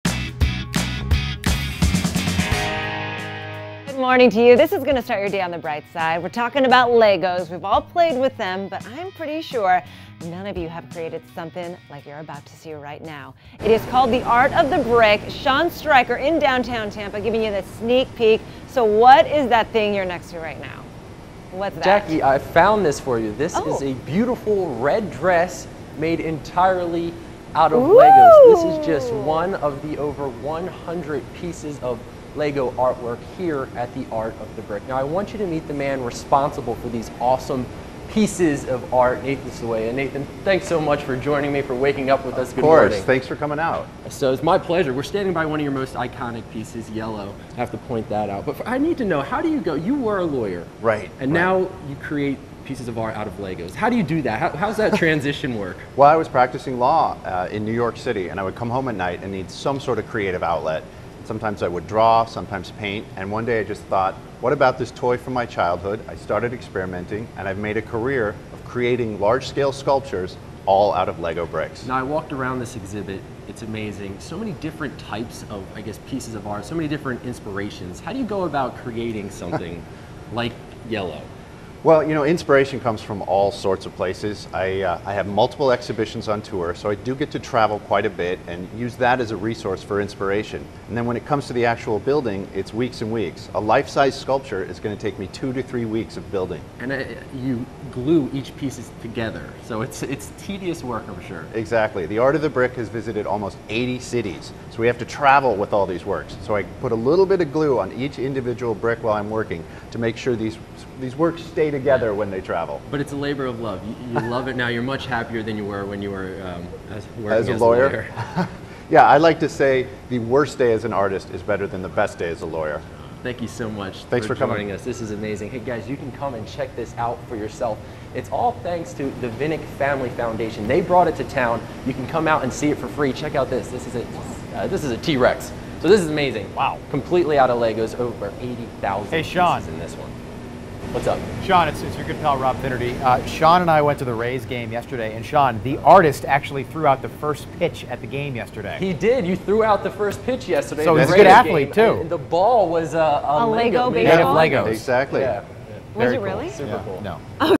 Art of Brick Lego exhibit comes to Tampa
"The Art of the Brick" debuts on June 23 at 802 East Whiting St. in Tampa. It runs through Sept. 4.